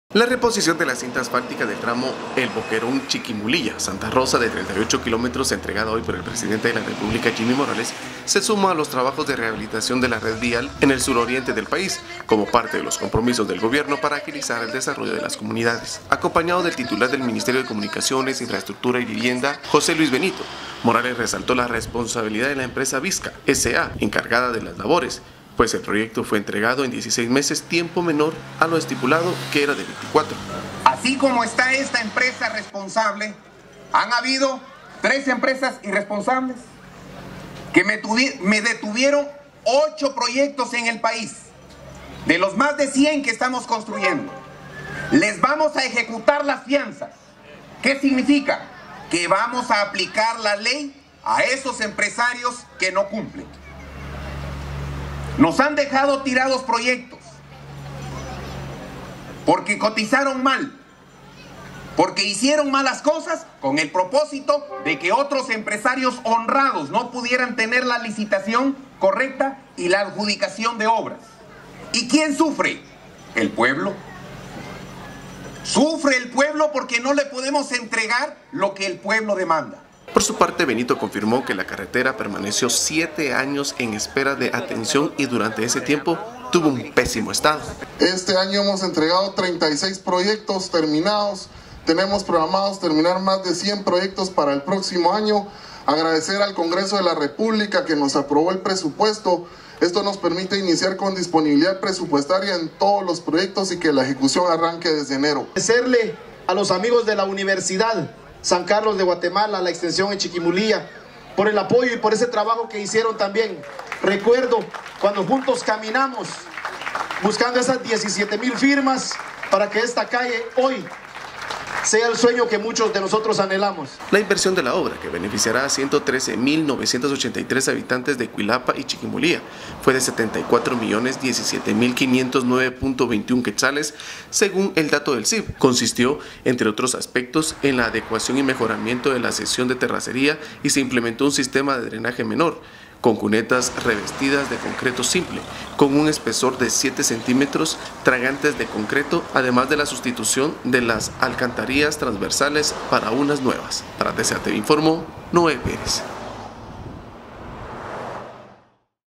Gobierno avanza en la rehabilitación de la red vial del suroriente
La reposición de la cinta asfáltica del tramo El Boquerón- Chiquimulilla, Santa Rosa, de 38 kilómetros, entregada hoy por el Presidente de la República, Jimmy Morales, se suma a los trabajos de rehabilitación de la red vial en el suroriente del país, como parte de los compromisos del Gobierno para agilizar el desarrollo de las comunidades.

Acompañado del titular del Ministerio de Comunicaciones, Infraestructura y Vivienda (CIV), José Luis Benito, Morales resaltó la responsabilidad de la empresa BICSA, S.A., encargada de las labores, pues el proyecto fue ejecutado en 16 meses, tiempo menor a lo estipulado, que era de 24.

El presidente Morales explicó que para el mejoramiento de la cinta asfáltica se hicieron trabajos en la base, las cunetas y las transversales de la carretera. “Se cumplieron todos los detalles y las garantías necesarias, y así poder dar cuentas a ustedes”, agregó.

El mandatario recordó que cuando se anunció el proyecto, el año pasado, un joven le dijo, sin malas intenciones, que esperaba que se cumpliera con la obra, “porque todos prometen pero nadie cumple”. En esa ocasión, el Presidente le respondió: “No te preocupes, yo sí terminaré lo que inicié”, y muestra de ello es que ahora los vecinos cuentan con una carretera en mejores condiciones.

Siete años sin hacer nada
Por su parte, Benito confirmó que la carretera permaneció siete años en espera de atención y que durante este tiempo estuvo en “pésimo estado”.

La inversión en la obra, que beneficiará a 113 mil 983 habitantes de Cuilapa y Chiquimulilla, fue de Q74 millones 17 mil 509.21, según datos del CIV. Consistió, entre otros aspectos, en la adecuación y mejoramiento de la sección de terracería, y se implementó un sistema de drenaje menor, con cunetas revestidas de concreto simple, con un espesor de 7 centímetros, tragantes de concreto, además de la sustitución de las alcantarillas transversales por unas nuevas.

La obra cumple con las políticas de desarrollo vial, contenidas en el Plan de Desarrollo Vial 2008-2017, de la Dirección General de Caminos, en 7 aspectos: integrar la CA-01 Oriente en El Boquerón a la cabecera municipal de Chiquimulilla; desarrollo rural con énfasis en la estructura básica; contribuir con el mejoramiento de los accesos pavimentados a las cabeceras municipales, y construir caminos de penetración a las aldeas y caseríos de Guatemala.

Además, desarrollar la red vial en la región (IV) suroriente del país; realizar inversiones viales en las regiones donde se remarque el índice de pobreza, y desarrollar la red vial que responda a lo establecido en los acuerdos de Paz.

*Con información de Berta Abrego